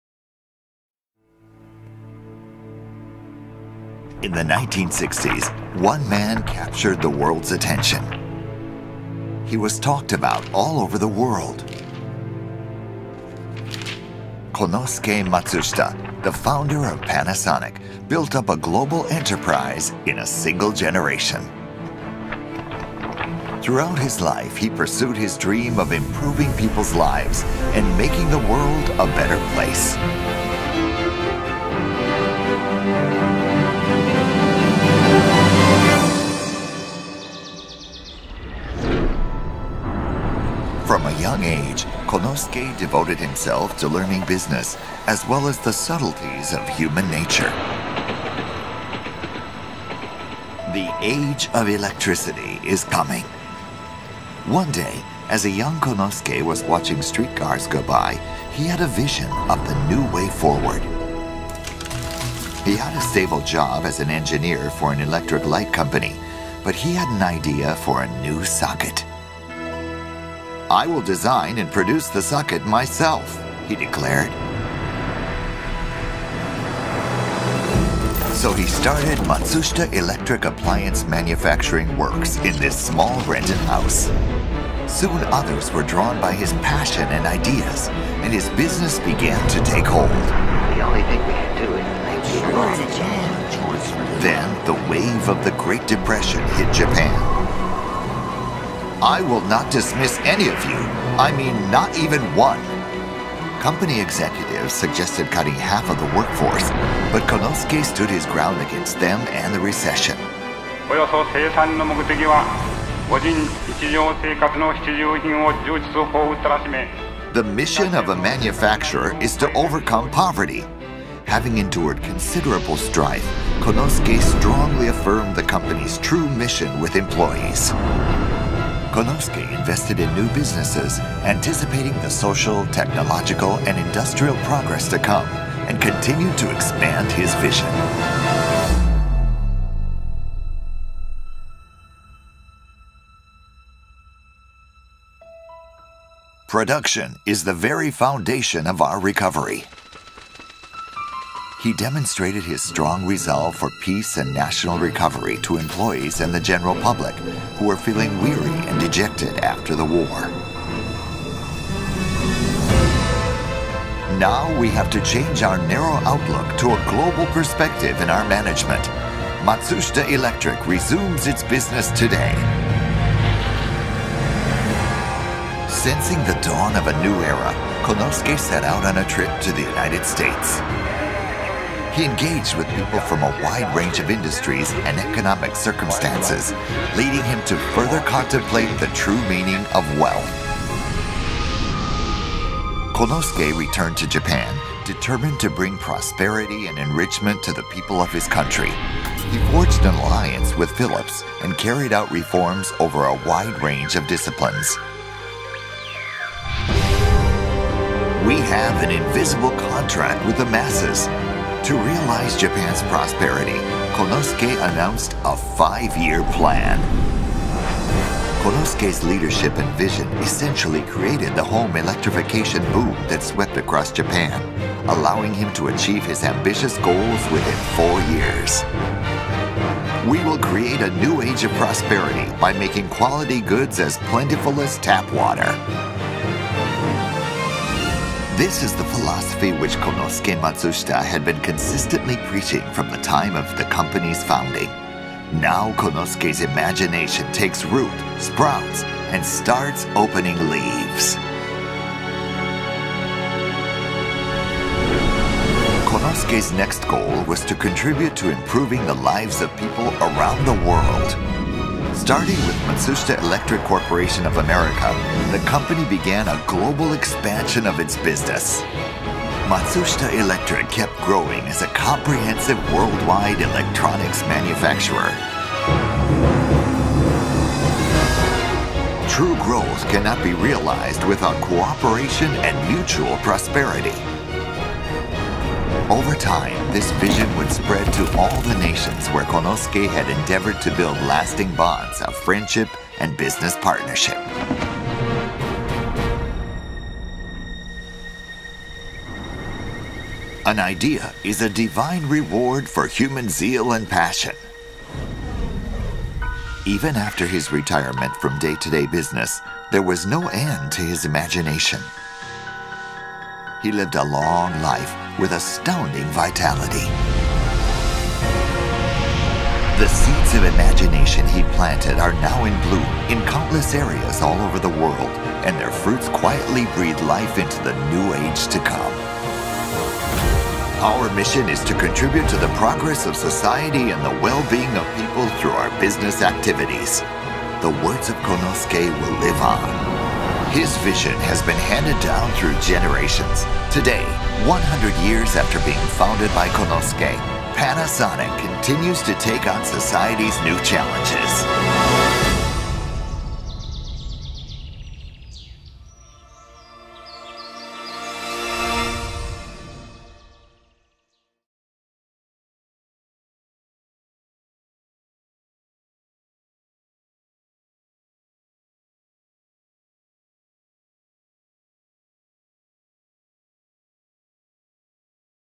Meet Mr Matsushita
At Panasonic, our roots are humble, traced back to the beliefs and actions of one man, Konosuke Matsushita, and his tireless
pursuit of ideas that would benefit the greater good. What began a century ago with 3 workers and the desire to build
a better light socket has grown to become a global leader with over 240,000 employees, with sales of more than $70
billion dollars a year.
for more from the show floor!